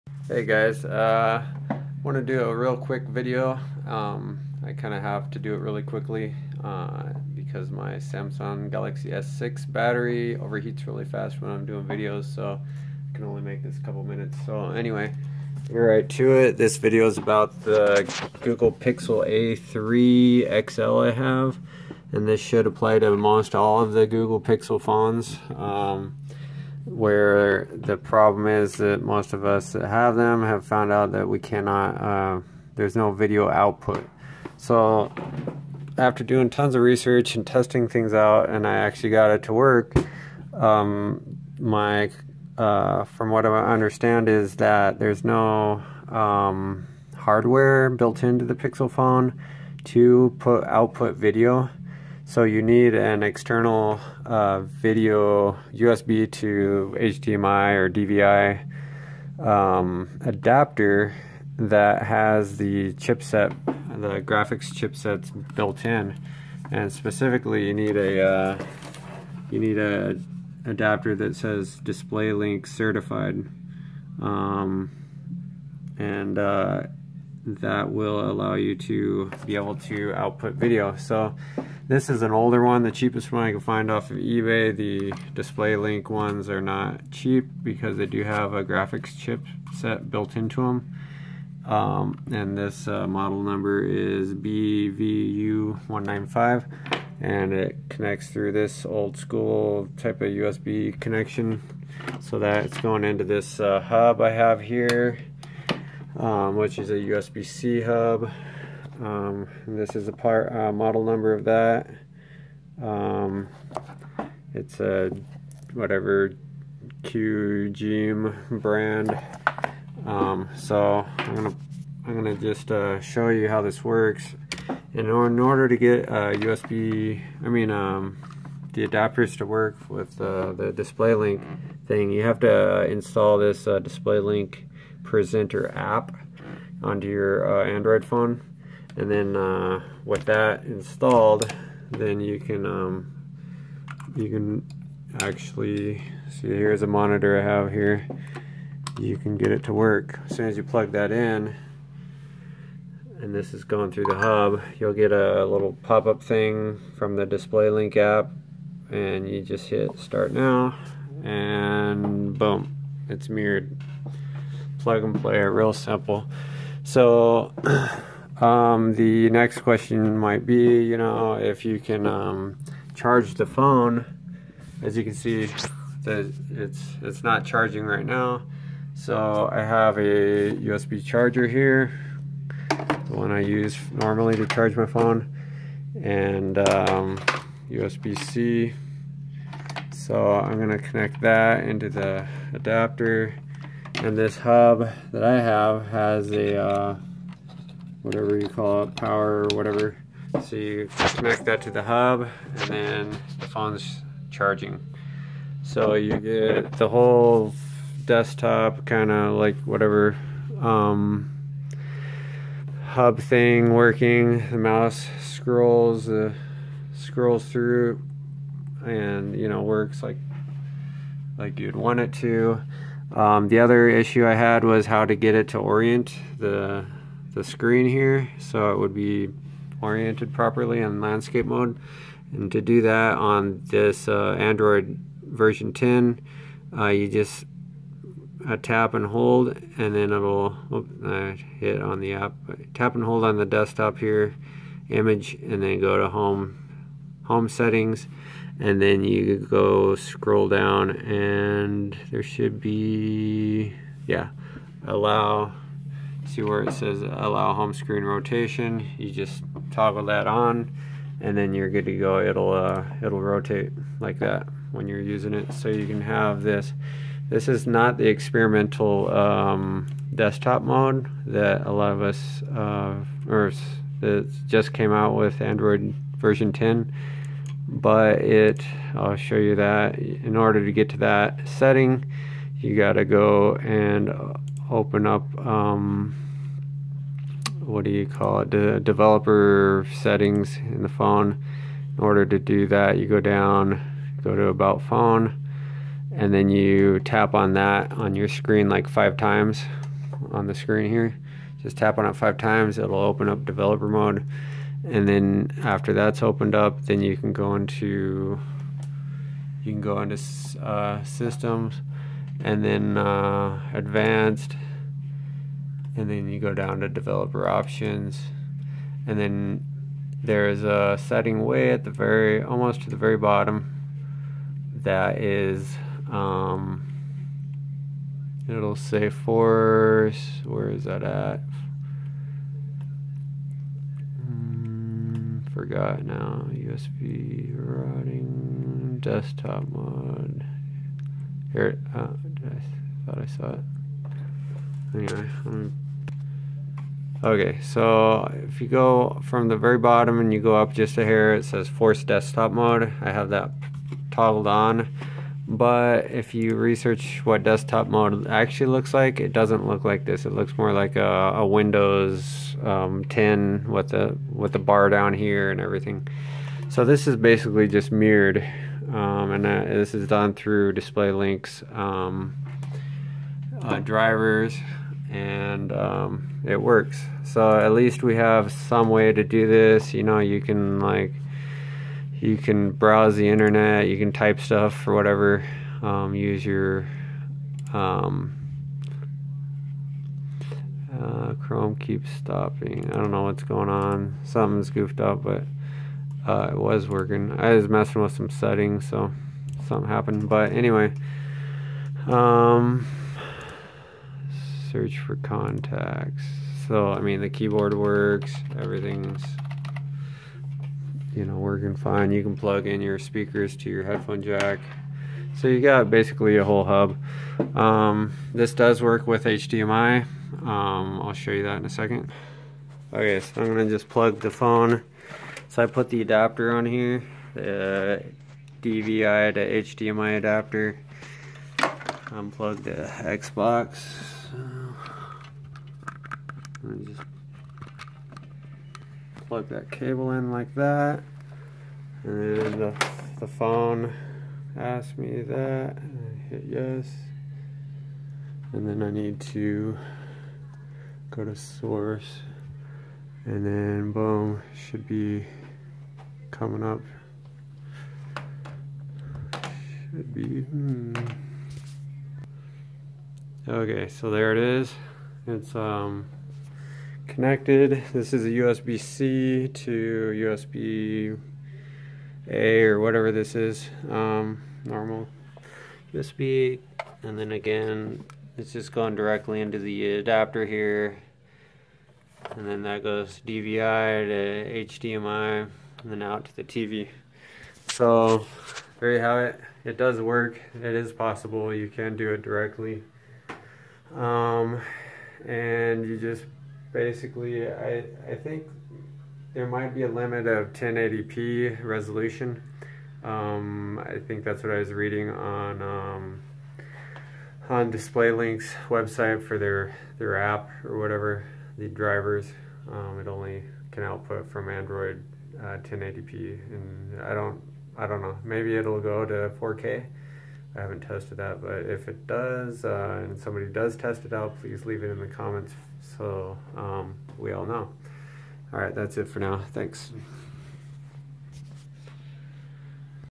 Pixel 3 to HDMI Video Output (using a WIRED adapter)
This video shows how to setup a hardwired (Not Wireless) HDMI video output on my Google Pixel 3a XL.  You need a DisplayLink Certified USB to DVI/HDMI adapter. And you need to install the DisplayLink Presenter app onto your Pixel phone. This should also work on the Pixel 2 Pixel 3 Pixel 4 & Pixel 4a phones.

This old 2nd Gen Display link chipset from 2007 that my adaptor uses in this video is a little laggy when playing games and HD videos full screen.   I suggest you buy an adapter with a newer chipset:
DL-5xxx chipset  - 2014  4K UHD, USB 3.0
DL-6xxx  chipset - 2016  4Kp60, USB-C or USB-A

Diamond (DisplayLink Certified) USB 2.0 to VGA  Adapter (with old DisplayLink DL-195 chipset) :

QGeeM 7 in 1 USB-C Hub:

DVI to HDMI Adapter:

USB 3.0 to USB-C adapter: